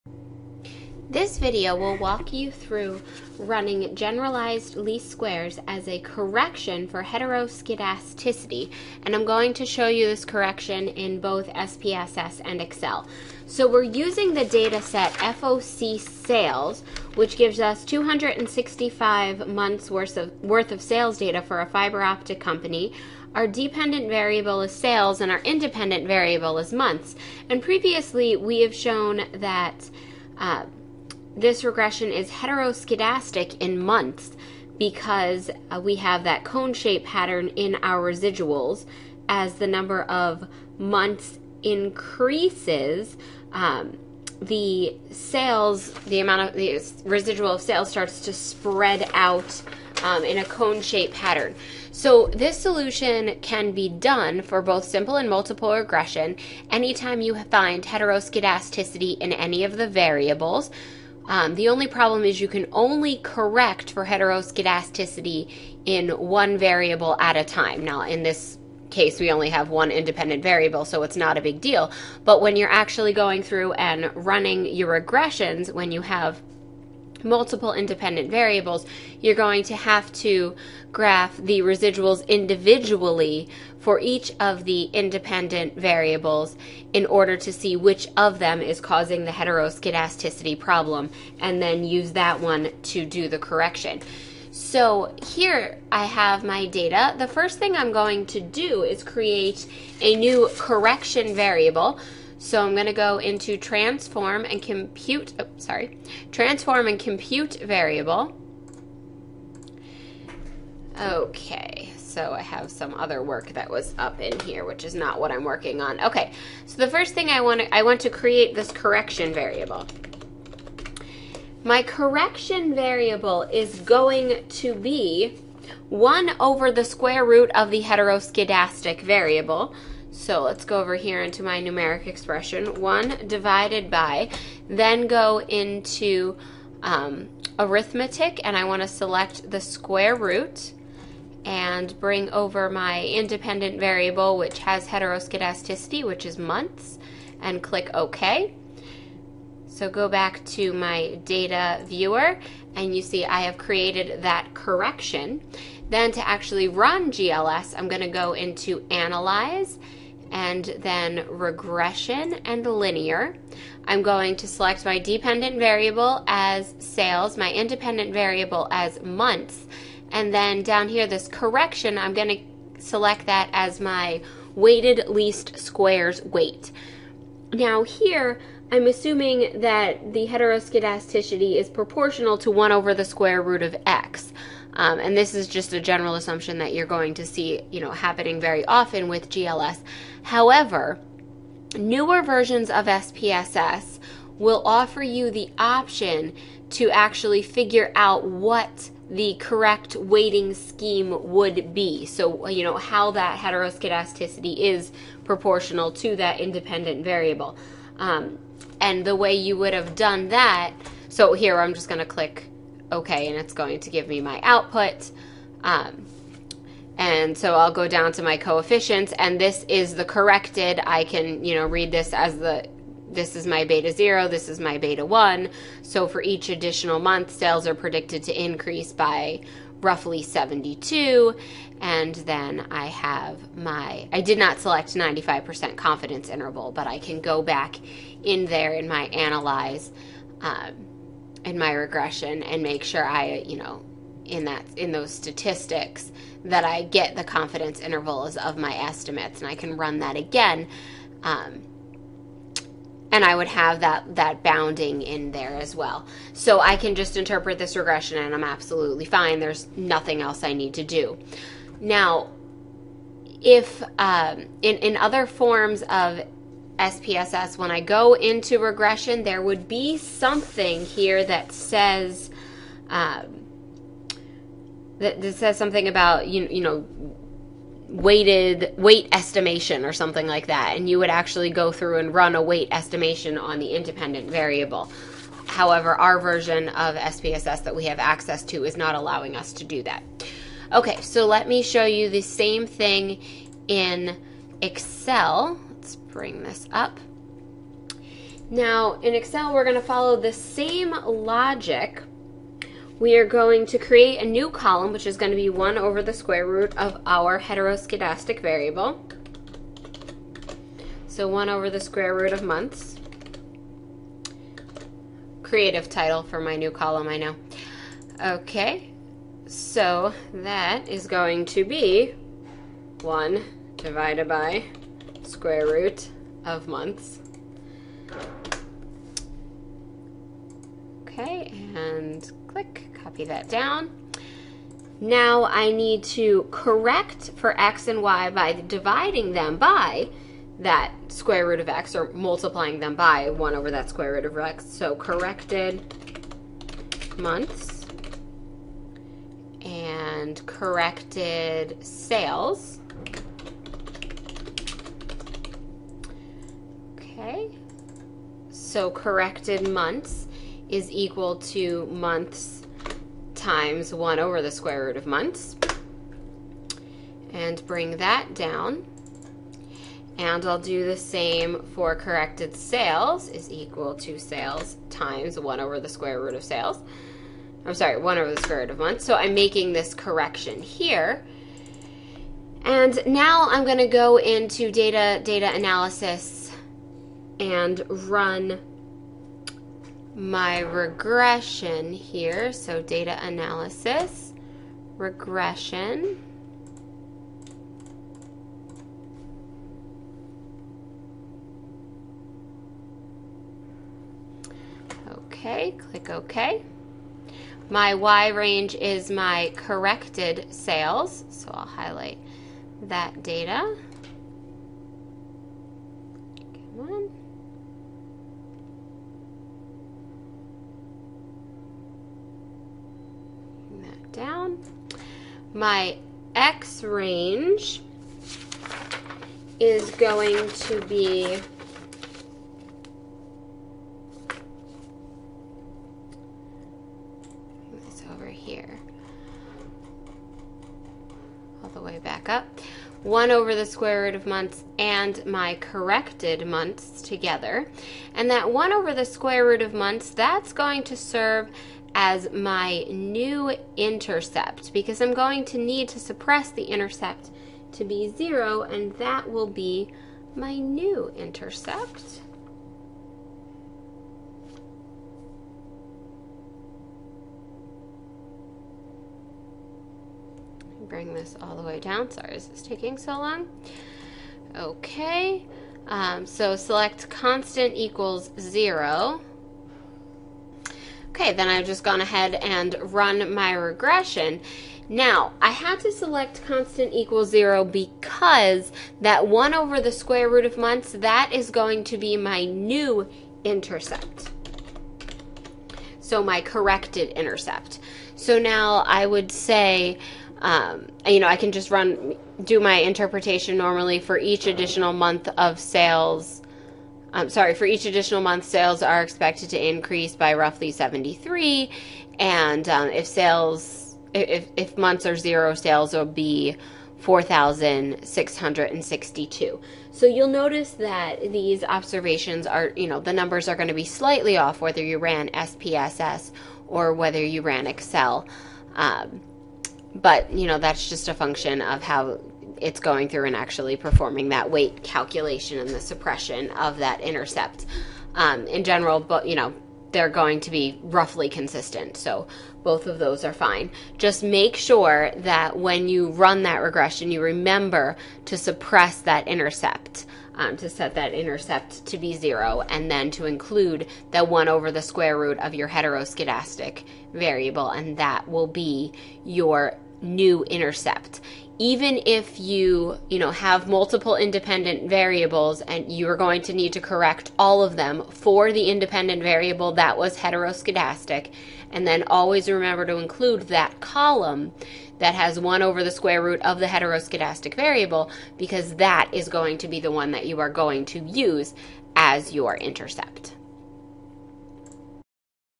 365, ch 6 hetero correction
This video demonstrates how to run  GLS to correct for heteroskedasticity (a violiation of assumption 2) using both Excel and SPSS. The esample used is the FOCSales example from the ch 6 slides.